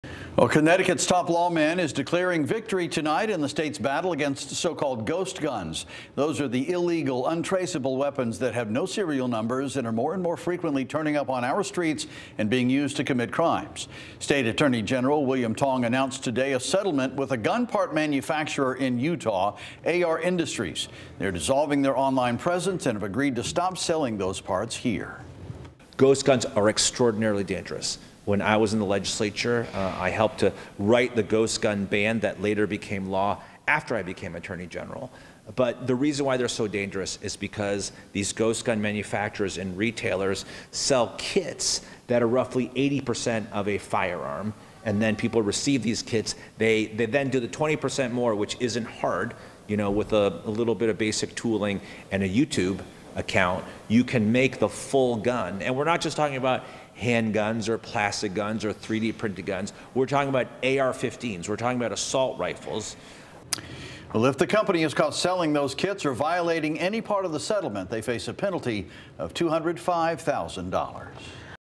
Utah-based AR Industries shut down by CT Attorney General William Tong for illegal ghost gun sales
AR Industries will cease operations after settling with the state following its sale of illegal untraceable ghost gun parts.